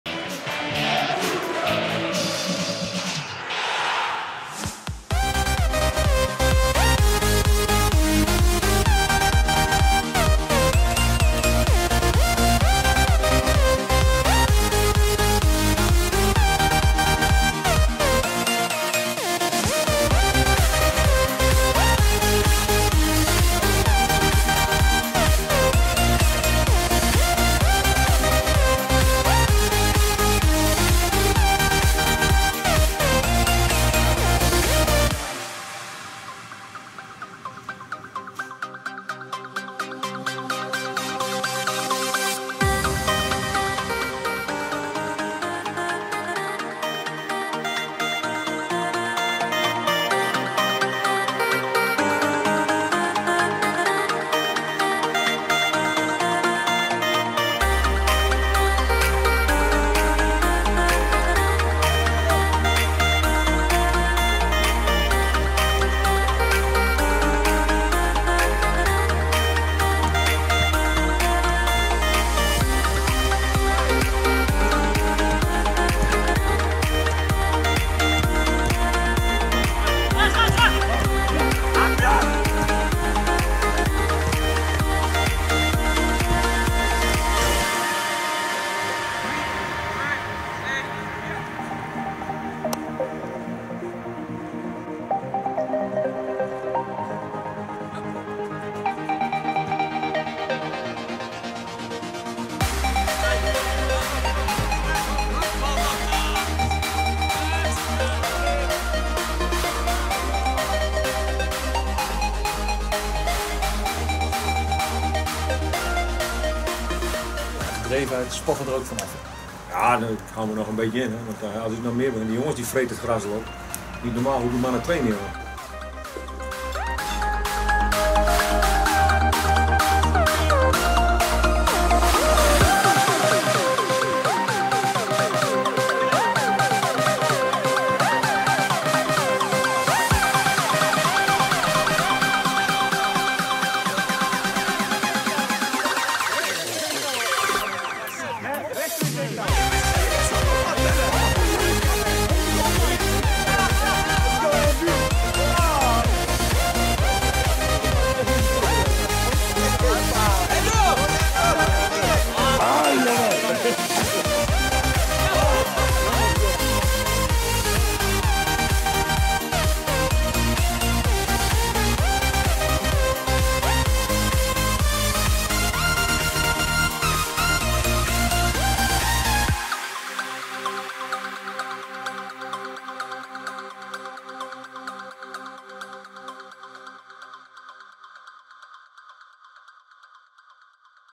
De Graafschap op trainingskamp bij KNVB Campus in Zeist
De Superboeren zijn 2017 begonnen met een kort trainingskamp op de nieuwe campus van de KNVB in Zeist. Onder leiding van  trainer Henk de Jong kon er onder uitstekende omstandigheden worden gewerkt aan het tweede deel van het seizoen. Naast hard werken, was er ook tijd voor een gesprek en een lach.
Wi-j komt altied weer terug! Zorg dat je er live bij bent.
Een compilatie van 3 minuten, zodat je weer helemaal up to date bent:)